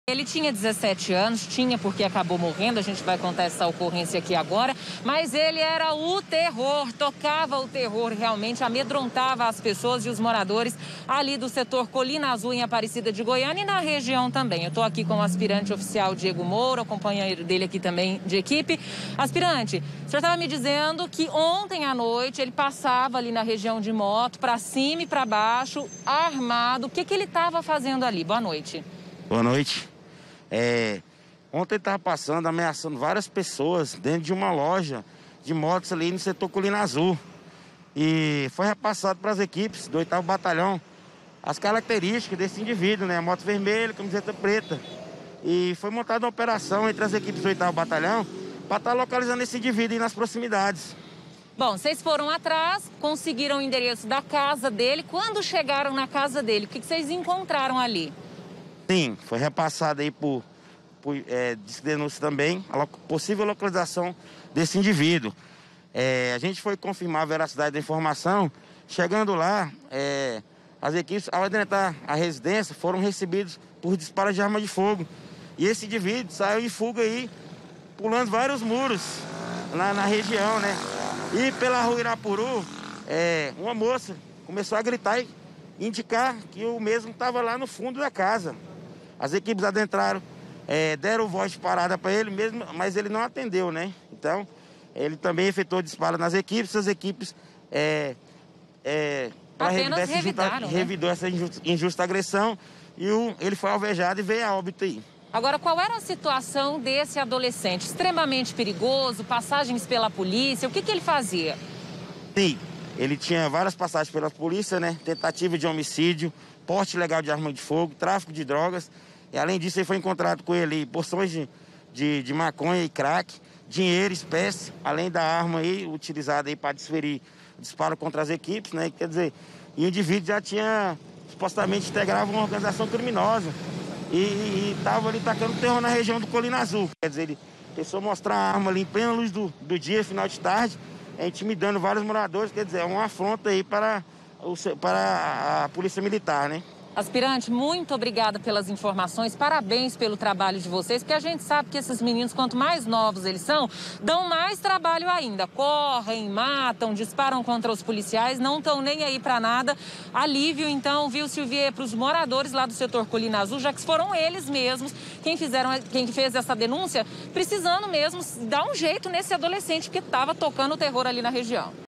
ADOLESCENTE MORRE EM TROCA DE TIROS
Jovem de 17 anos morre em troca de tiros com a polícia.